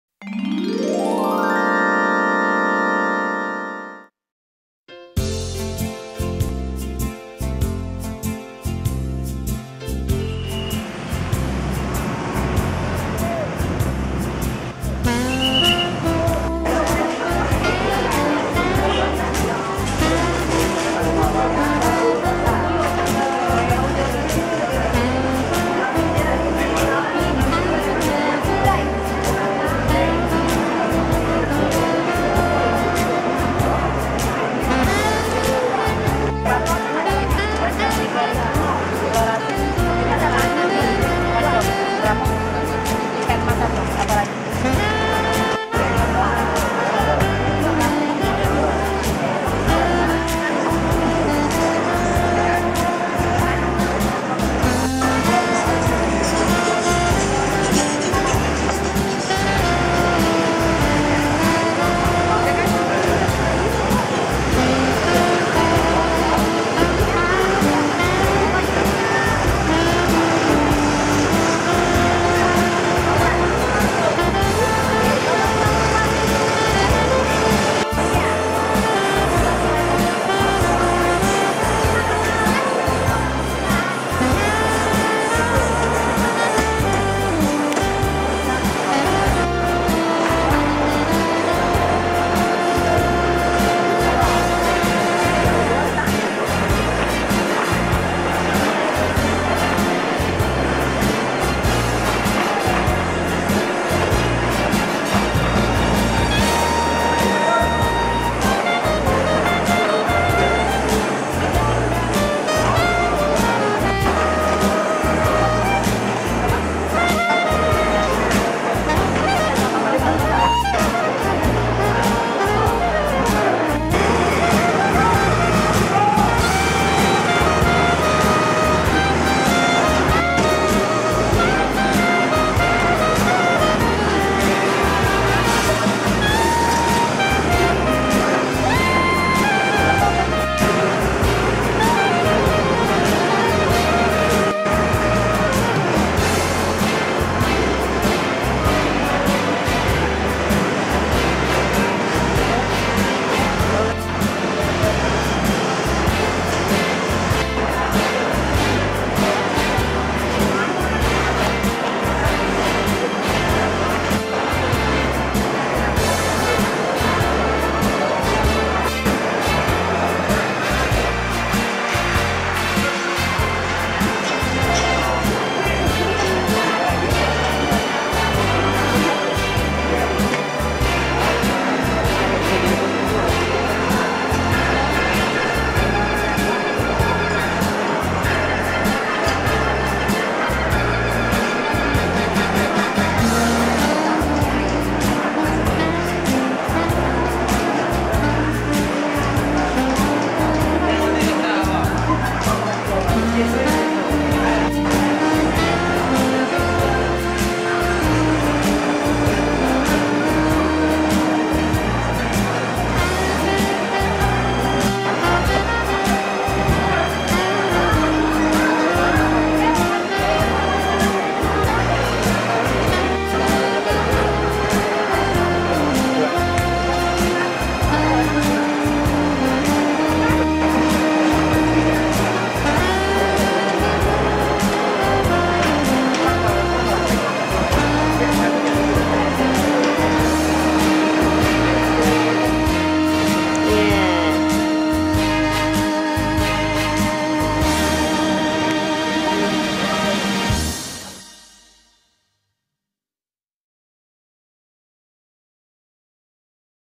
Travel Indonesia-Jakarta BSD CITY Market Place 在印尼椰城参观当地人的菜市场
Local market place at Jakarta BSD CITY. Clean and organized. Very good impression.
到当地人的菜市场看看，物品种类繁多，空气流通，干净整齐。印象很好。